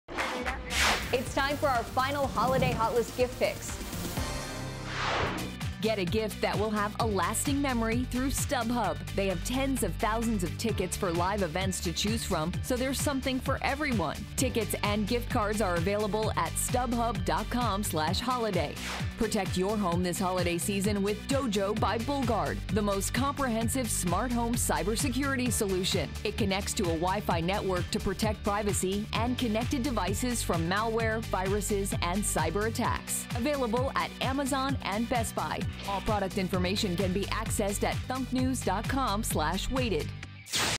Holiday Hotlist with Stub Hub and More...
Host Sonia Isabelle with gift ideas from Stub Hub and Dojo by Bullguard Smart Home Security System...